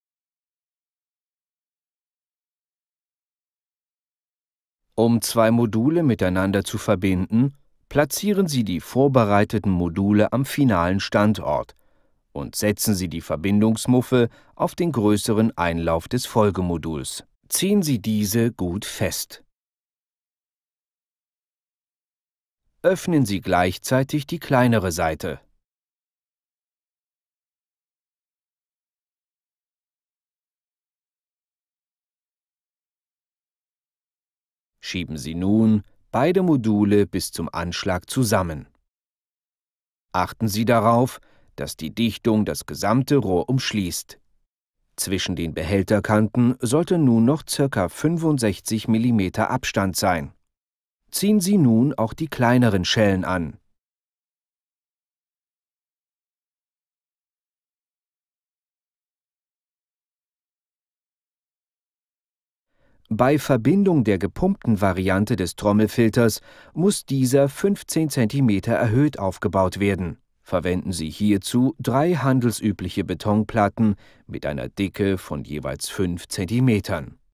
OASE | Trommelfilter - Modulverbindung herstellen ProfiClear Premium Installationsanleitung
OASE ProfiClear Premium - High-End Modulfiltersystem für Koiteiche und Großgewässer